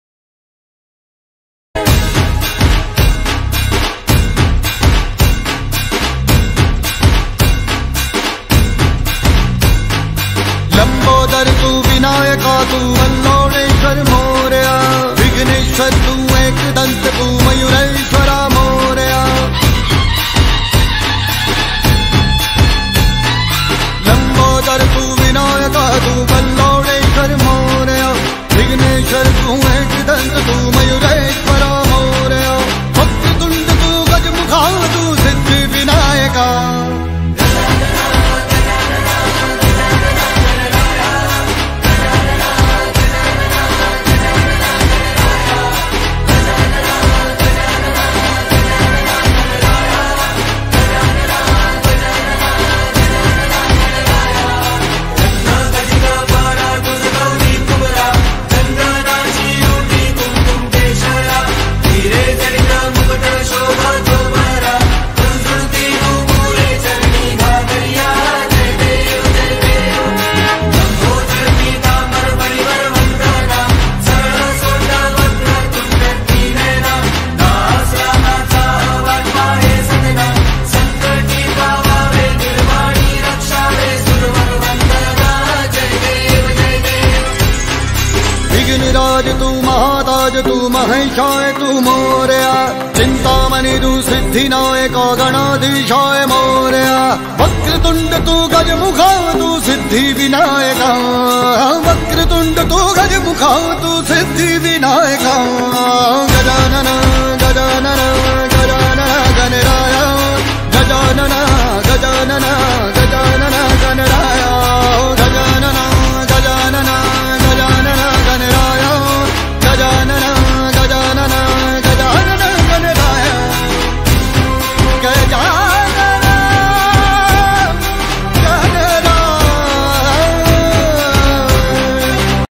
आज पर्यंत तुम्ही कधीही न पाहिलेला बोटांचा गणपती बाप्पा | Hand fingers Ganapati bappa
This is a beautiful "Ganapati Bappa" with Hand Fingers.
It is made on hand fingers ganpati bappa.😊

Material Required..

2) Brushes And Acrylic colours